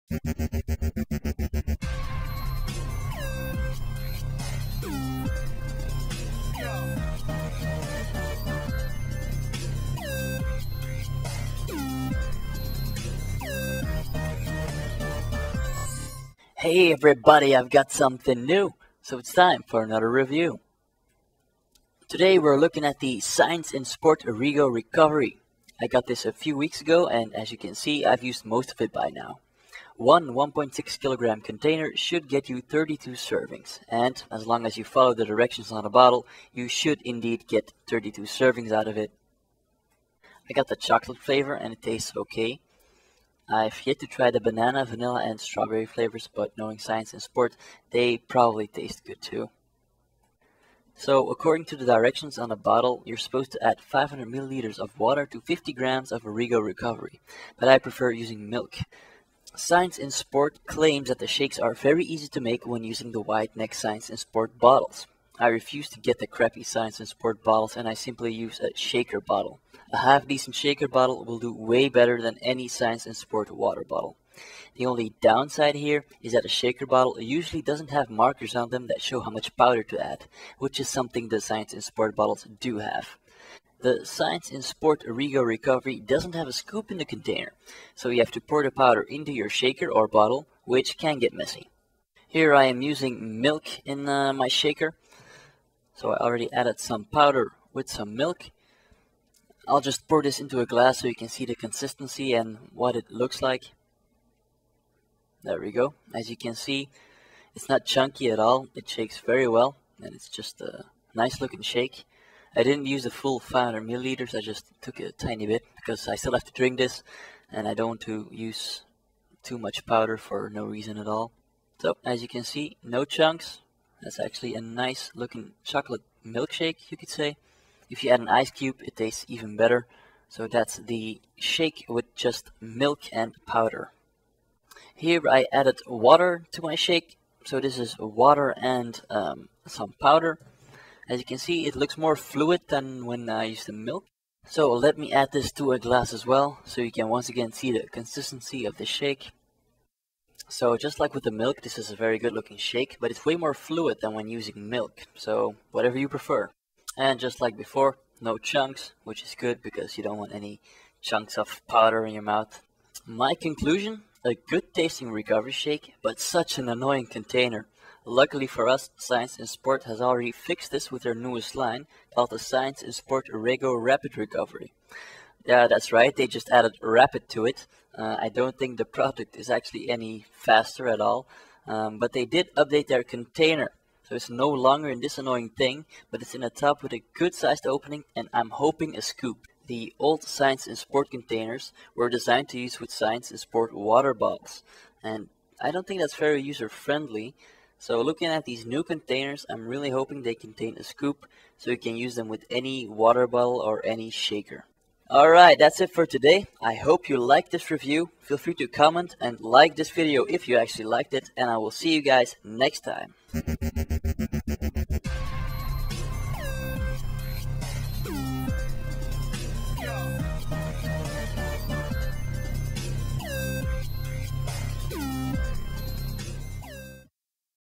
Science In Sport Rego Recovery Fuel REVIEW
My review of the Science In Sports Rego Recovery Fuel.

Ups:
Good taste; I reviewed the chocolate flavour but since SIS is known for their good tasting products, I am sure the others taste just as good.
It works; as a recovery shake, that is. This is not a meal replacement or weight gainer! You're supposed to take a shake after a training and have a decent meal afterwards. You will have less aches in the morning, if used correctly.
Decent price; look around online for the best prices. You shouldn't be paying more than €36 for a 1.6KG tub.

Downs:
Comes in an annoying container. SIS has fixed this in their newest line of recovery products (the SIS Rego Rapid Recovery). It now comes in a container with a wide opening and probably a scoop. If you order this right now (June 14. 2012), chances are you will still be sent the old version as shops are trying to get rid of their old stock.

Conclusion:
Good tasting stuff that actually works and you can get it at a decent price (around €1,1 per shake with a 1.6KG tub). Don't be afraid to go for the 1.6KG tubs right away, whatever flavour you pick, this goes down very well.
Bottle says to use water, but milk works as well. It's an easy way to get in a bit more protein if you need it (and I like the taste better with milk).